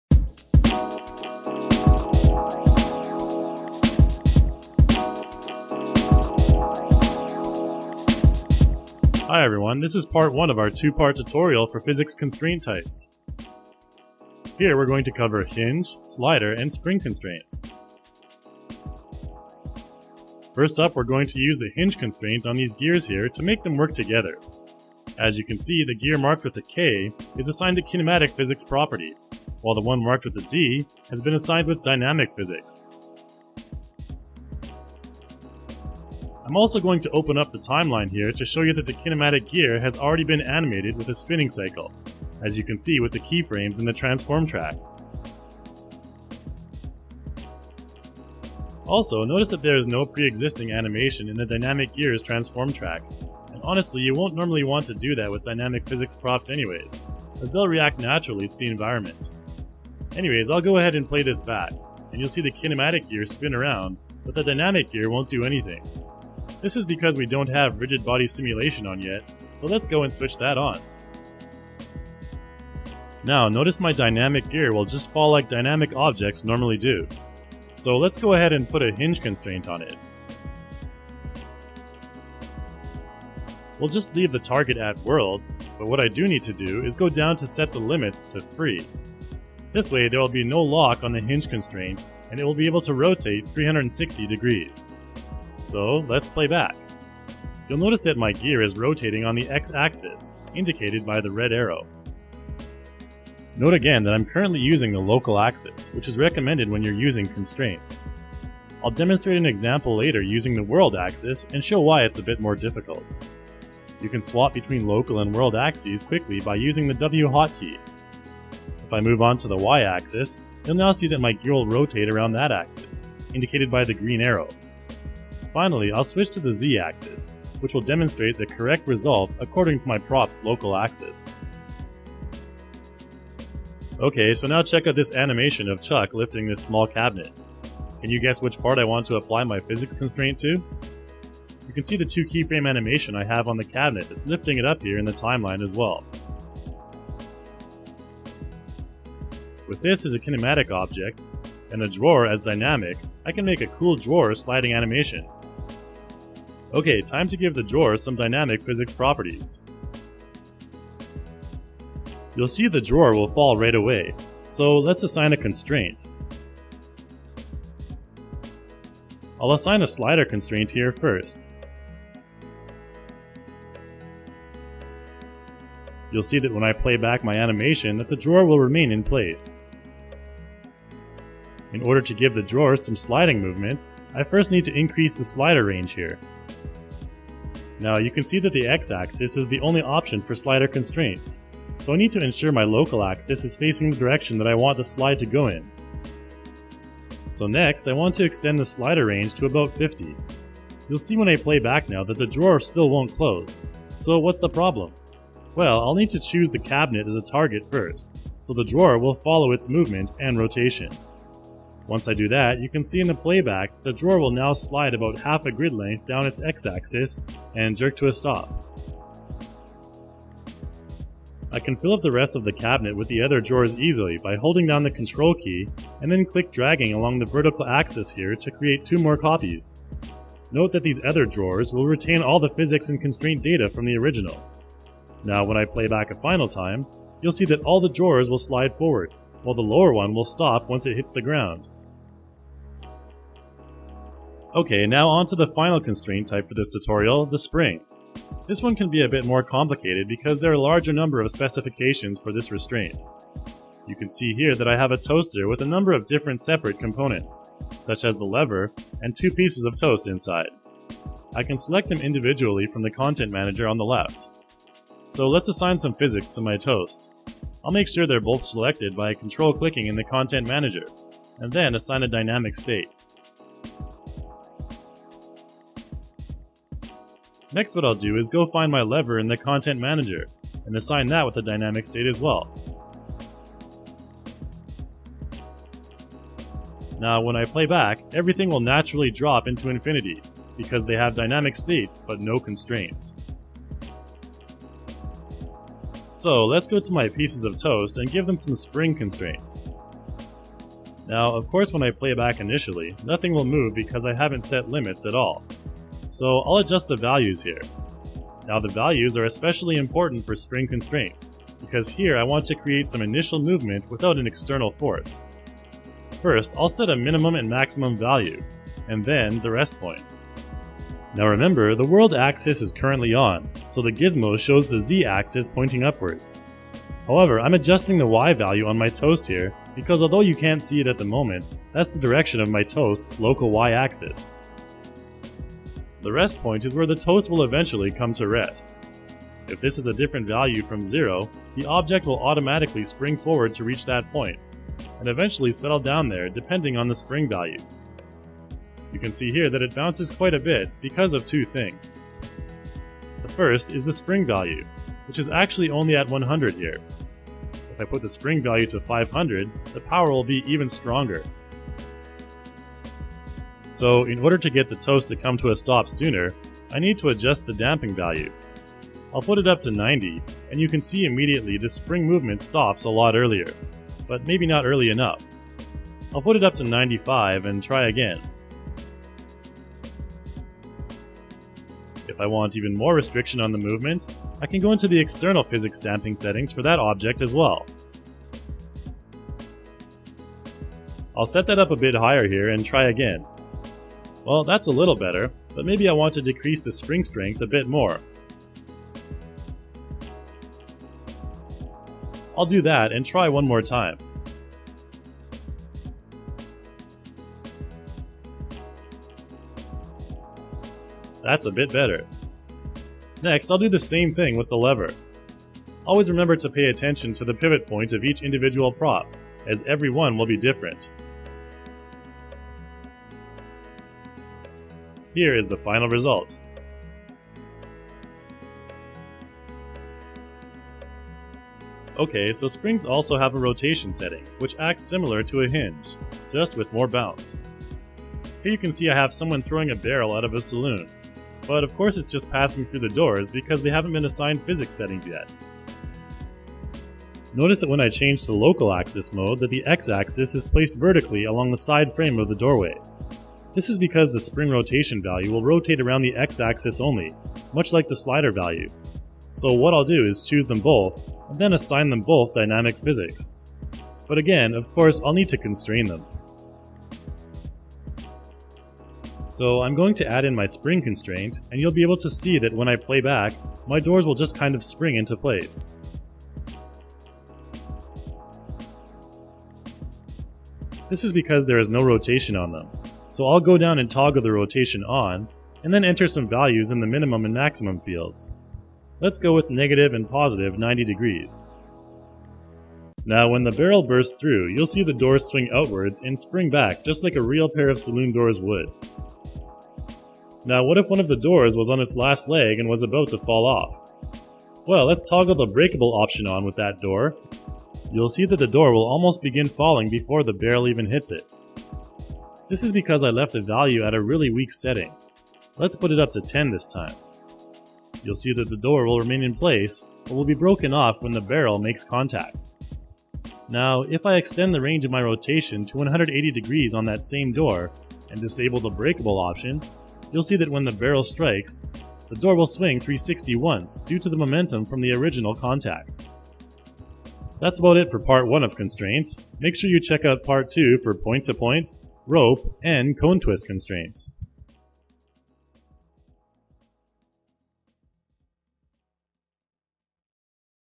iClone5 Tutorial - Constraint Types Part 1
This tutorial covers three of the basic physics constraint types in iClone 5: Hinge, Slider, and Spring.  This video will run through three different projects, each with a different constraint type.  You'll learn how to incorporate and customize these different constraint types, as well as how you can adjust and refine them to suit your project needs.

*Statement:
Some features in this video tutorial are for iClone5 PRO version only. For version comparison, please visit: